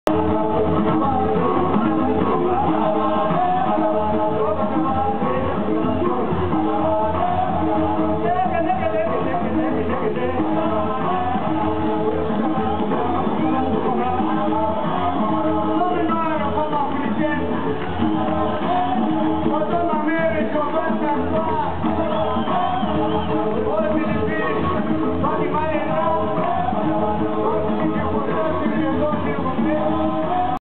Kasaï Allstars @ FMM Sines 2009
Concerto de dia 23 de Julho - Festival Músicas do Mundo, Sines, Castelo